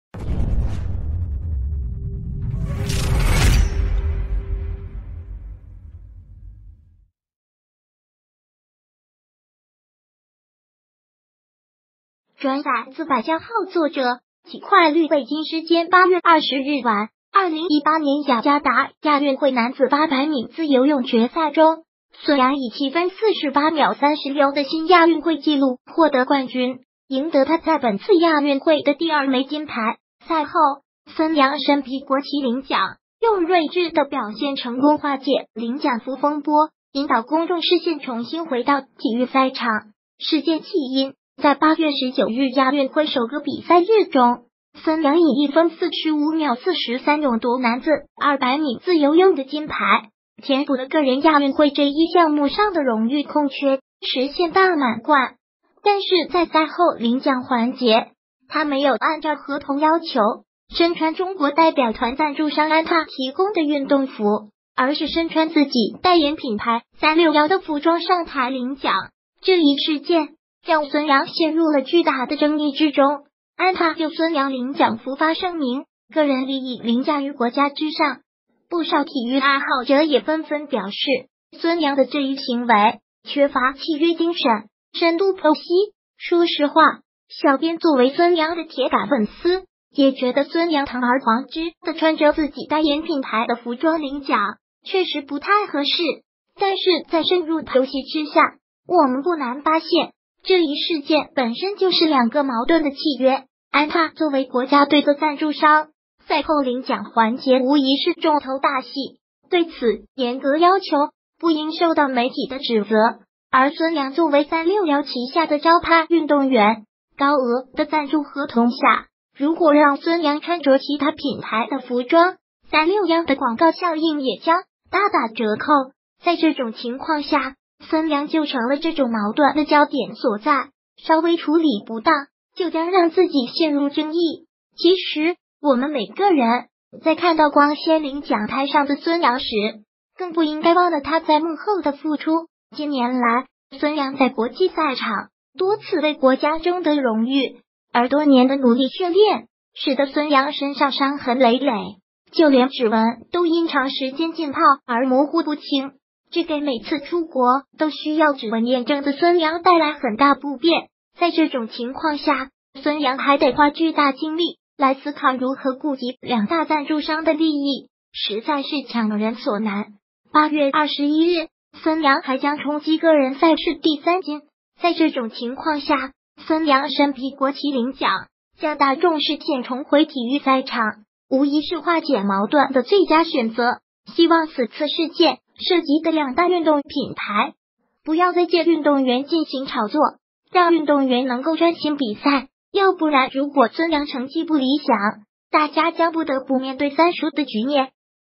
国旗才是最美领奖服，孙杨睿智表现让视线重回赛场
转载自百家号作者：几块绿北京时间8月20日晚，2018年雅加达亚运会男子800米自由泳决赛中，孙杨以7分48秒36的新亚运会纪录获得冠军，赢得他在本次亚运会的第二枚金牌。赛后，孙杨身披国旗领奖，用睿智的表现成功化解“领奖服”风波，引导公众视线重新回到体育赛场。事件起因：在8月19日亚运会首个比赛日中，孙杨以1分45秒43勇夺男子200米自由泳的金牌，填补了个人亚运会这一项目上的荣誉空缺，实现“大满贯”。但是在赛后领奖环节，他没有按照合同要求，身穿中国代表团赞助商安踏提供的运动服，而是身穿自己代言品牌361的服装上台领奖。这一事件，让孙杨陷入了巨大的争议之中。安踏就孙杨领奖服发声明:个人利益凌驾于国家之上。不少体育爱好者也纷纷表示，孙杨的这一行为，缺乏契约精神！深度剖析：说实话，小编作为孙杨的铁杆粉丝，也觉得孙杨堂而皇之地穿着自己代言品牌的服装领奖，确实不太合适！但是在深入剖析之下，我们不难发现，这一事件本身就是两个矛盾的契约！安踏作为国家队的赞助商，赛后领奖环节无疑是重头大戏，对此严格要求不应受到媒体的指责；而孙杨作为361旗下的招牌运动员，高额的赞助合同下，如果让孙杨穿着其他品牌的服